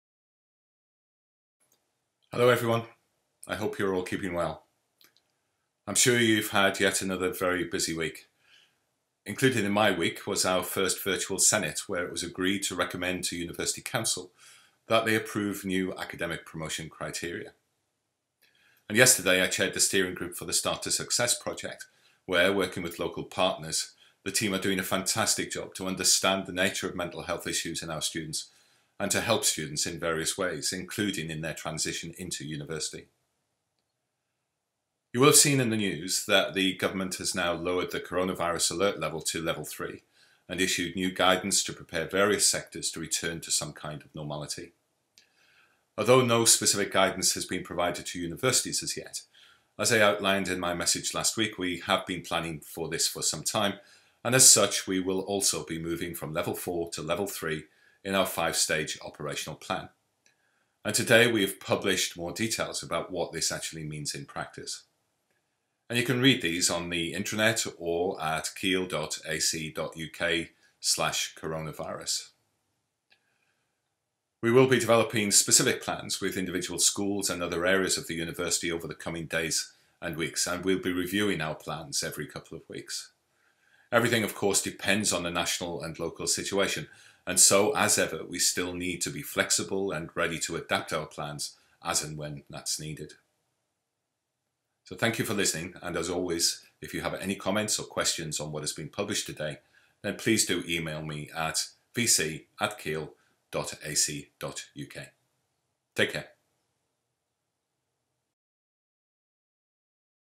Covid-19: Update from Professor Trevor McMillan, Vice-Chancellor
Last week the UK coronavirus alert level was lowered, and the government outlined new guidance to support the re-opening of many parts of the economy and civic life from July onward.

Although no specific guidance has been provided to universities as yet, as I outlined in my message last week we have been planning for this for some time and as such we too will be moving from level four to level three on our five-stage operational plan as of today, and I have outlined below our approach to transitioning to this level of operations.